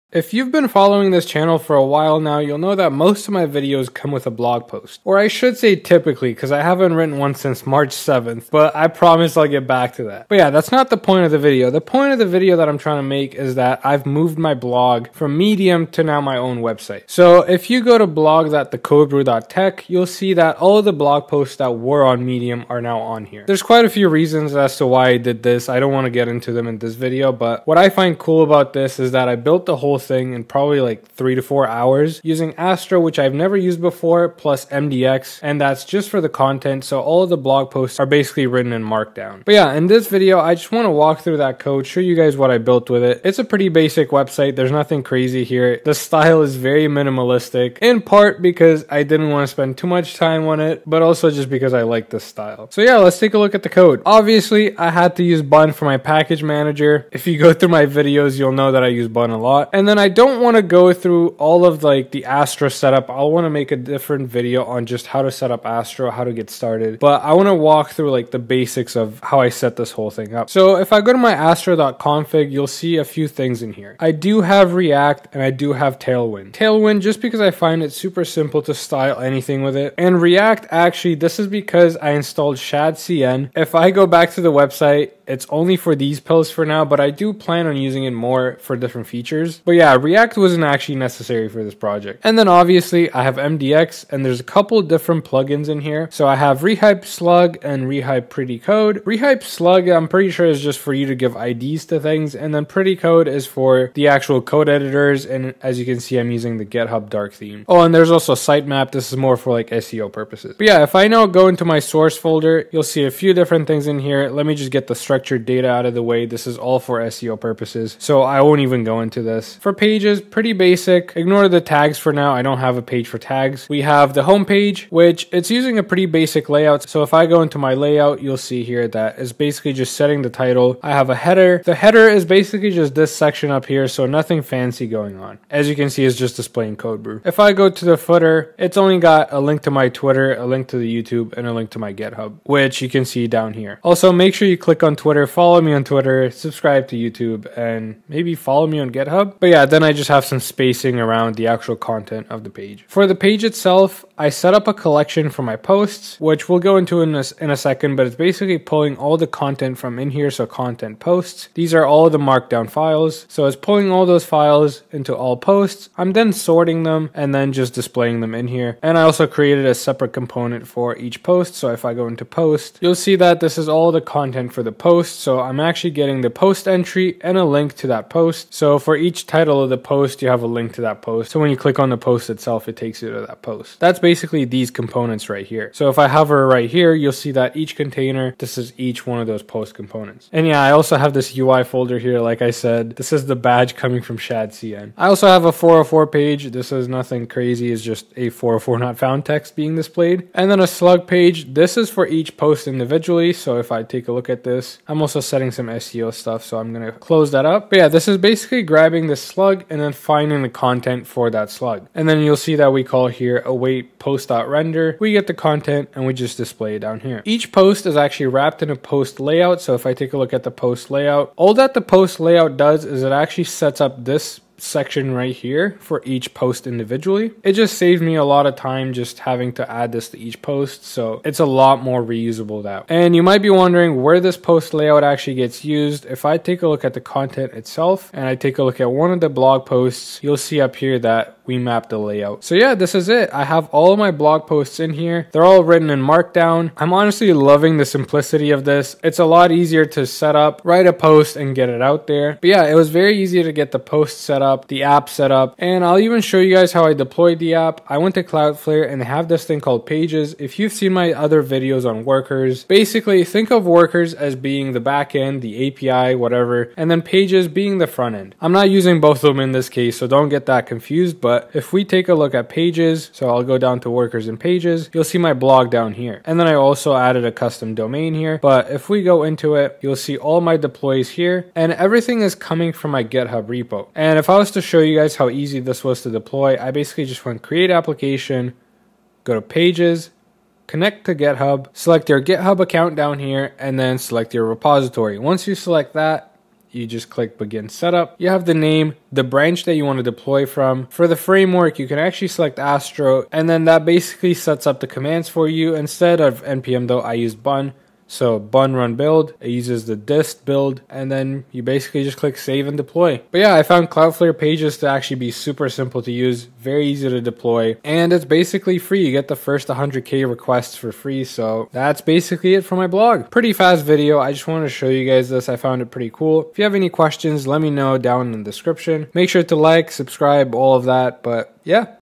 How I Built My Blog Using Astro & MDX
Decided to move my blog from Medium to building my own using Astro and MDX then self hosting it using Cloudflare Pages! Wanted to make a short video walking everyone through the code and how the deploy process is. TLDR: Super simple!

Let me know if you would be interested in a video doing a deeper dive into Astro and possibly Astro DB.